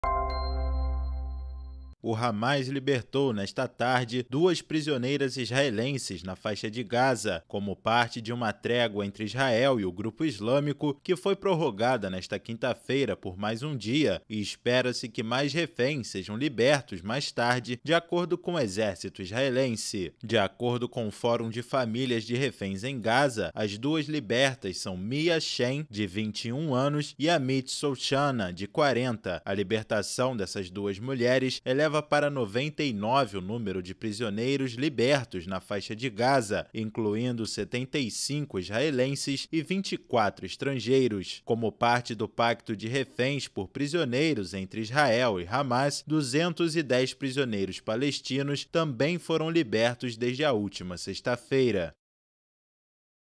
Hamas solta duas israelenses e deve libertar outros reféns mais tarde
Jerusalém, 30 nov (EFE).- O Hamas libertou nesta tarde duas prisioneiras israelenses na Faixa de Gaza, como parte de uma trégua entre Israel e o grupo islâmico que foi prorrogada nesta quinta-feira por mais um dia, e espera-se que mais reféns sejam libertos mais tarde, de acordo com o Exército israelense.
IMAGENS CEDIDAS